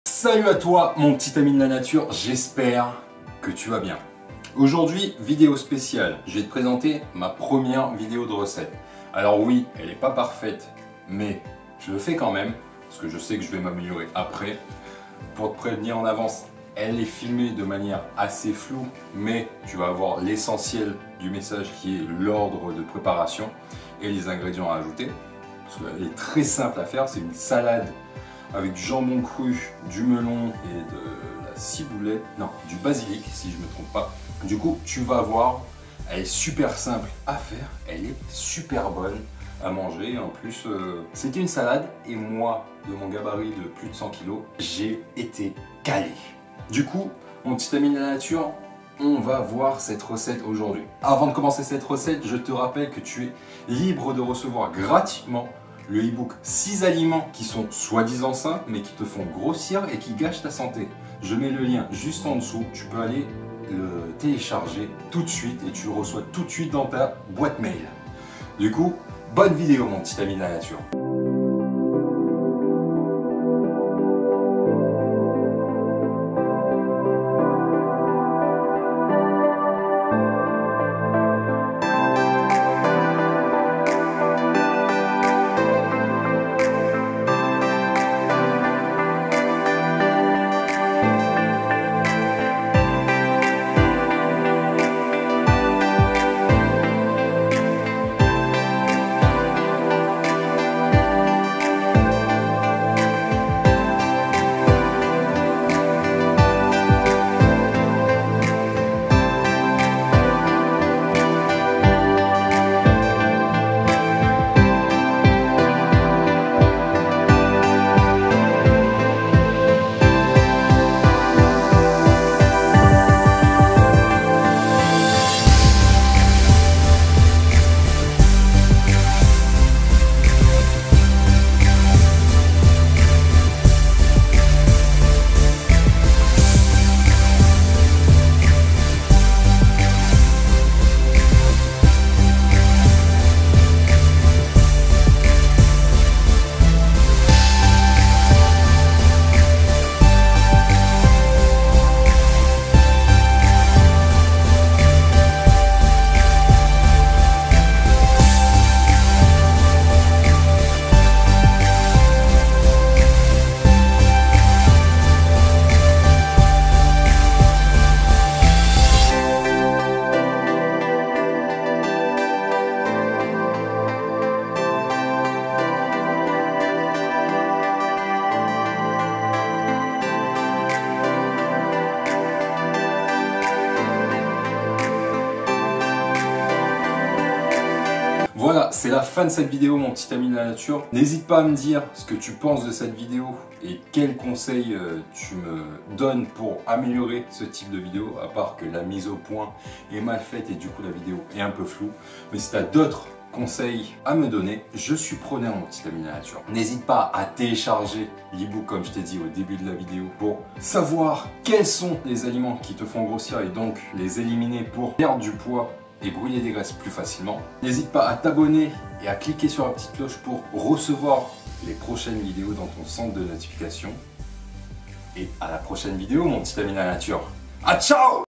Recette paleo salade au melon jambon cru et basilic
Voilà une recette paleo de salade au melon et jambon à tester !

INGREDIENTS:
200g de melon
1 poivron jaune
1 poivron vert ( les poivrons sont remplaçable par du concombre pour les personnes qui n'aiment pas le poivron)
80g à 120g de jambon cru (idéal sans nitrites)
Graines de courge
Feuilles de basilic
1 poignée de roquette
1/2 citron
Vinaigre de cidre
Huile d'olive